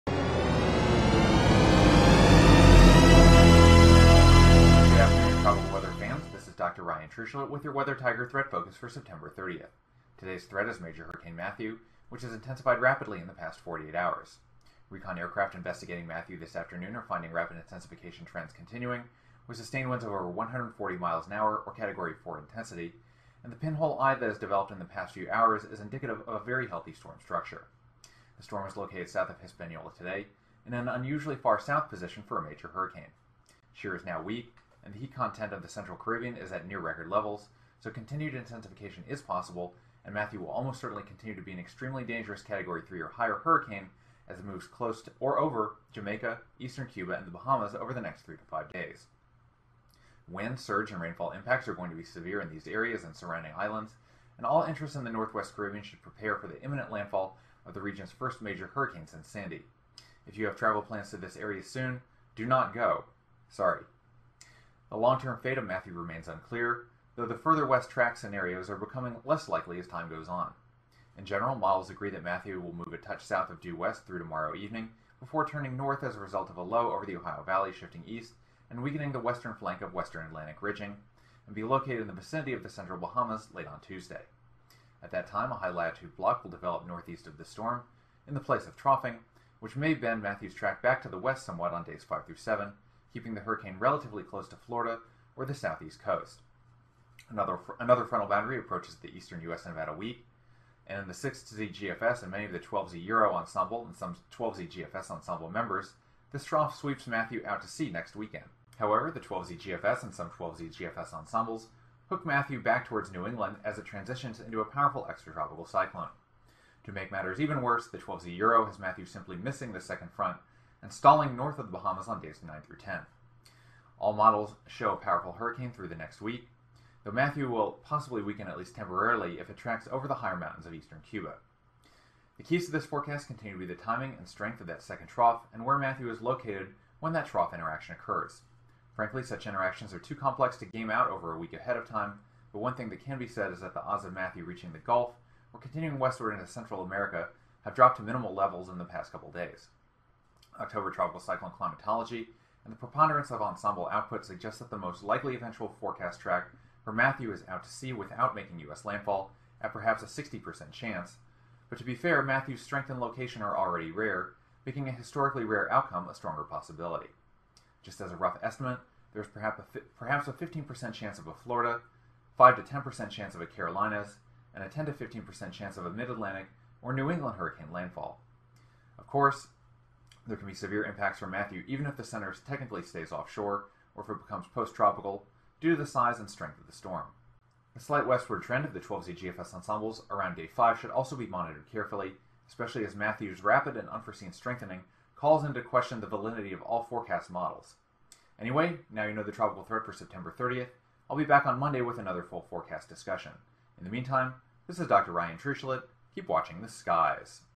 Category 5 Hurricane Matthew Forecast for September 30th and October 1st
•Matthew has become an extremely dangerous category 5 hurricane in the central Caribbean Sea with maximum sustained winds from recon of 160mph. This system will continue as a powerful major hurricane towards Jamaica, eastern Cuba, and the central Bahamas over the next 3-4 days.

•Matthew remains a major concern for the continental U.S. in the 6-10 day range. Odds have diminished of a track into the Gulf of Mexico, but a range of outcomes from a track out-to-sea to a landfall in southeast Florida or north along the U.S. East Coast in the Carolinas or New England remain possible. Overall, I believe that it is slightly more likely (60%) than not that Matthew will not directly make U.S. landfall, but this idea is certainly subject to change and this is a lower than average confidence forecast.

•Matthew will be a large and powerful storm, so even if there is not a direct landfall, the hurricane could have significant impacts along the full extend of the U.S. East Coast. Southern Florida will likely, at the least, see showers and gusty winds starting on Tuesday as Matthew is moving over the Bahamas.